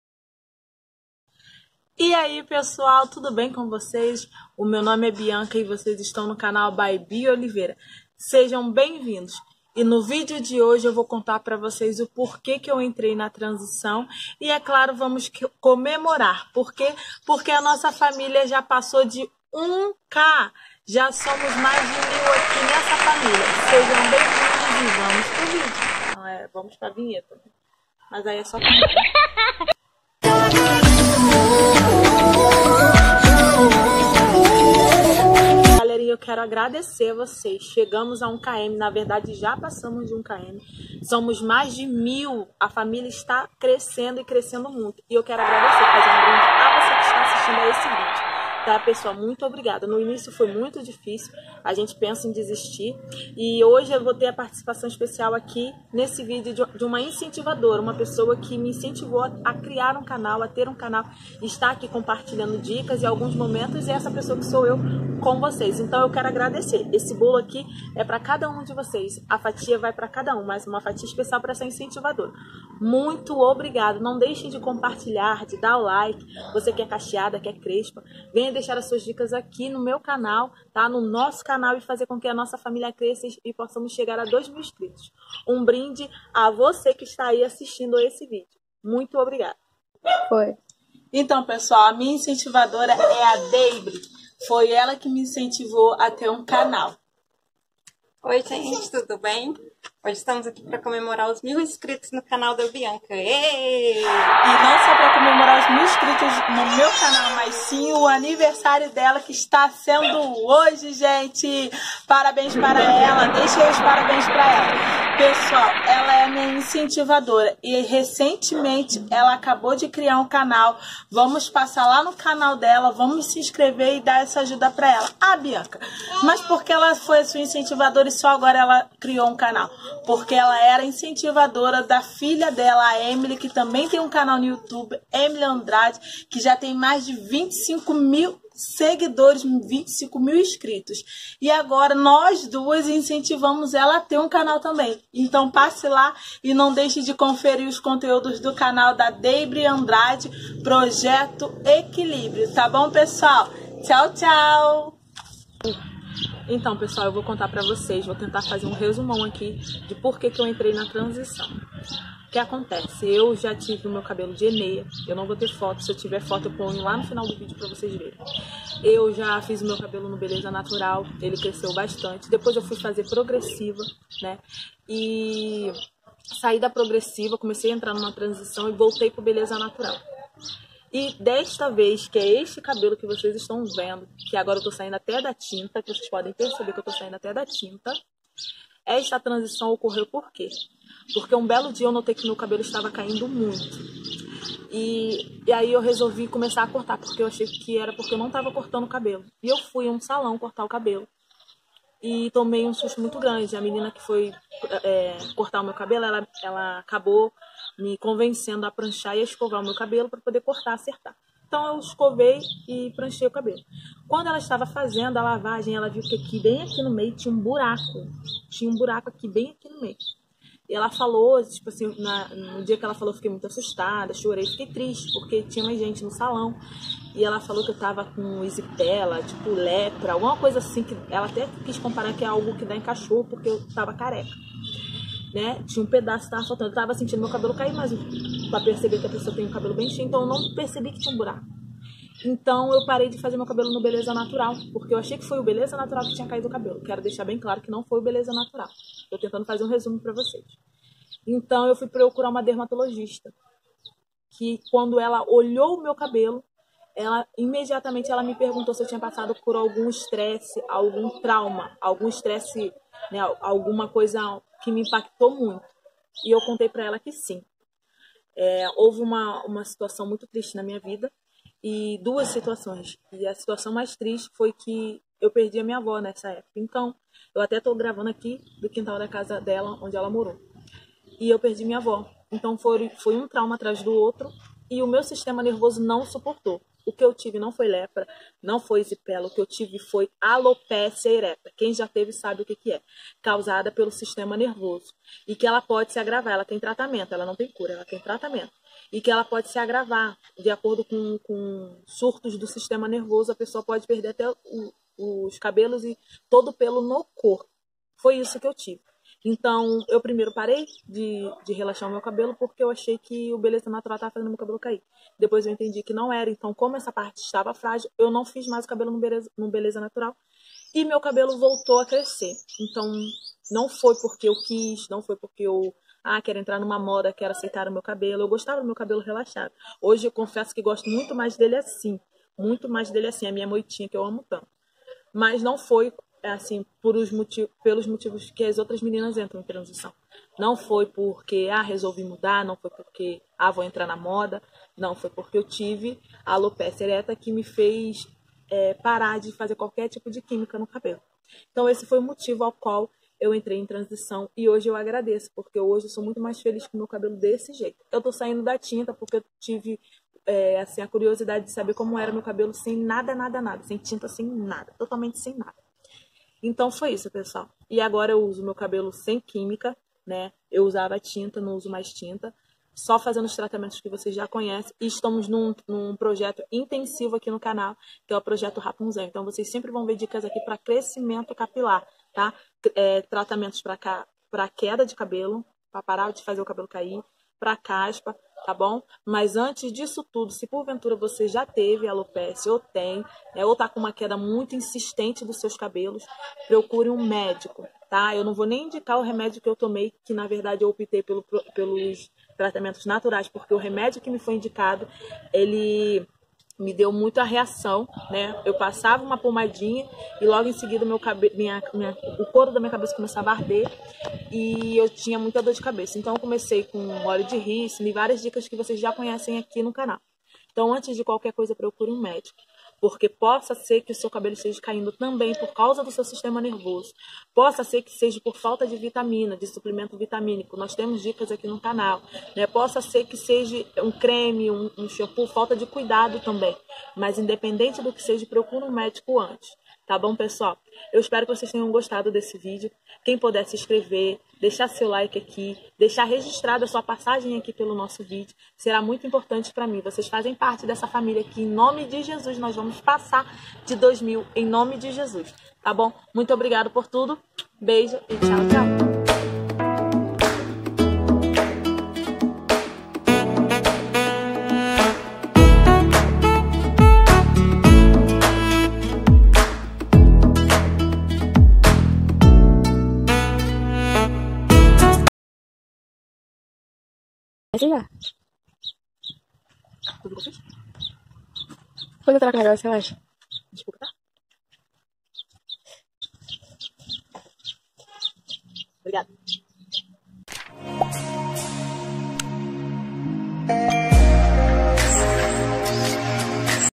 MINHA TRANSIÇÃO CAPILAR | ESPECIAL MIL INCRITOS
Oiiiii bençãos! Neste vídeo conto o porquê entrei na transição capilar, e comemoro os primeiros mil incritos nesta família abençoada! Muito obrigada a todos vocês !!